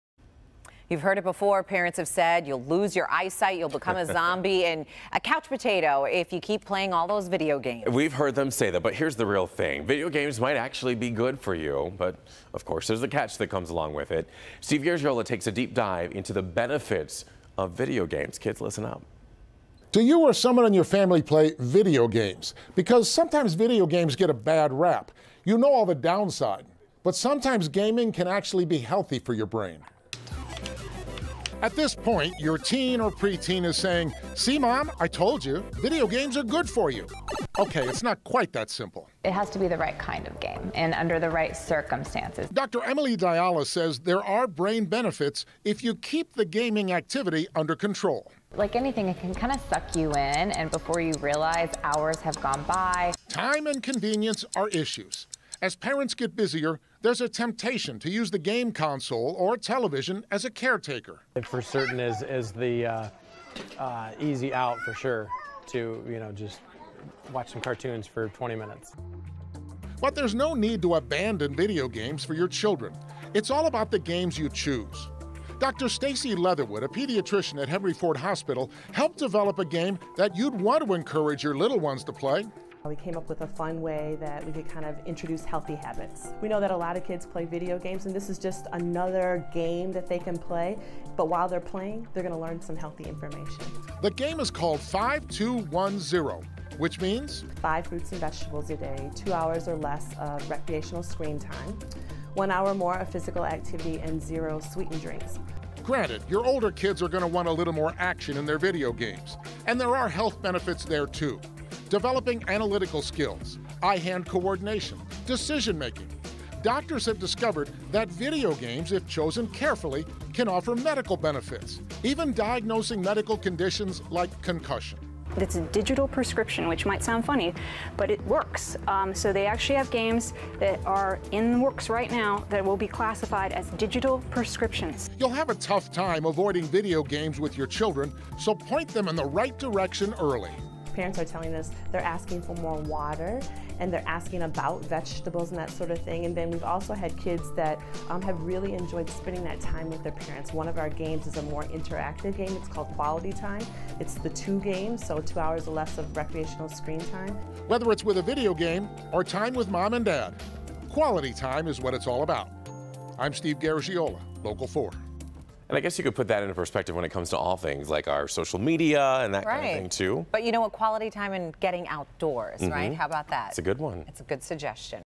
Experts say video games can actually have health benefits
Parents have told children for years that they'll lose their eyesight and become couch potatoes if they keep playing video games, but experts say video games can actually be good for people.